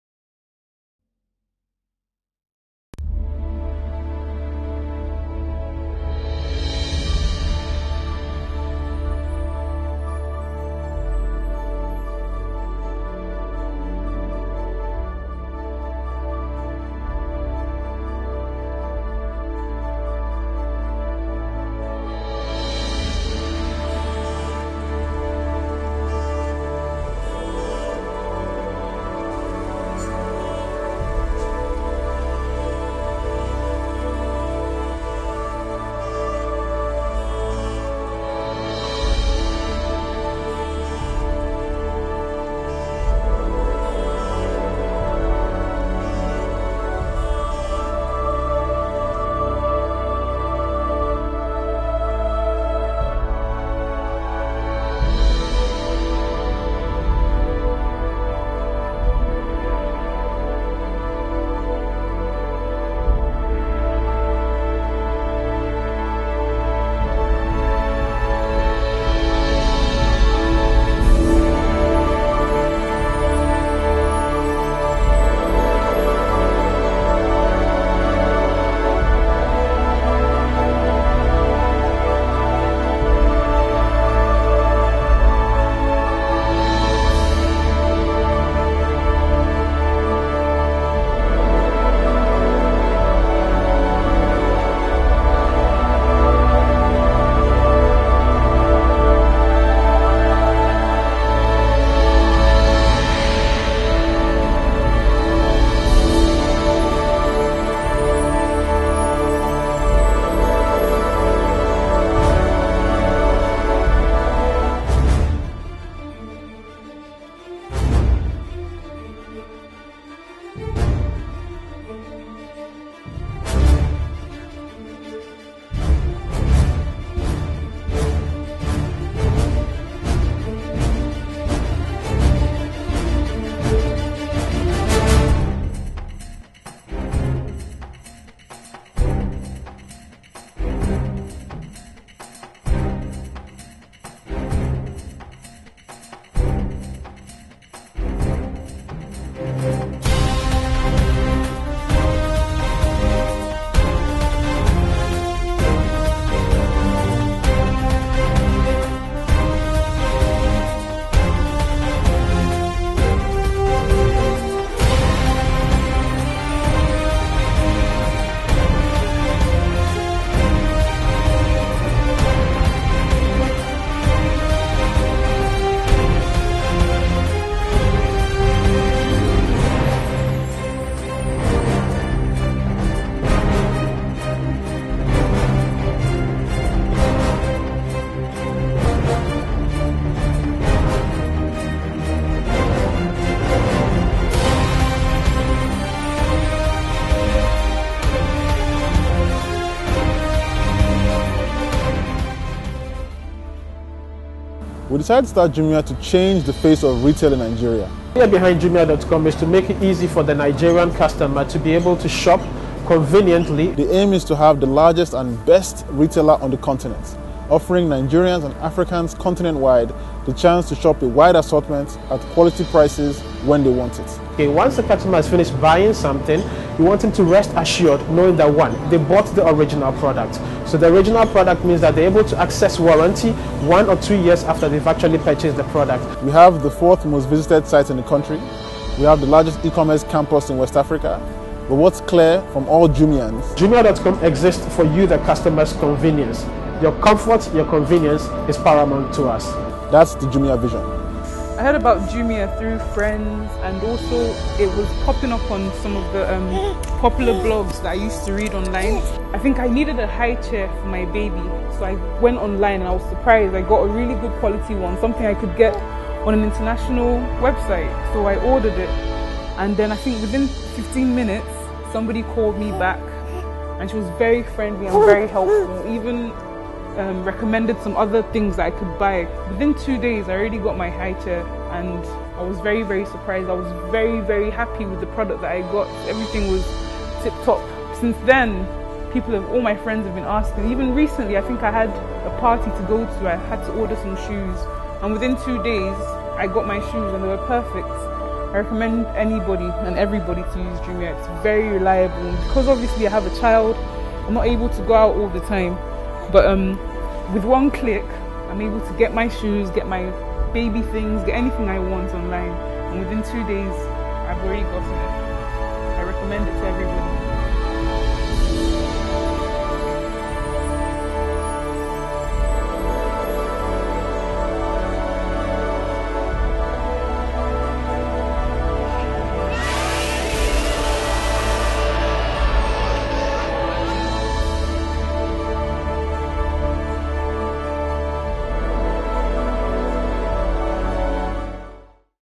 The Jumia Story
Jumia.com.ng, Nigeria's No1 online shopping destination recently celebrated her first year anniversary. We put a short video together to tell the story of how we've brought about significant change to Nigeria.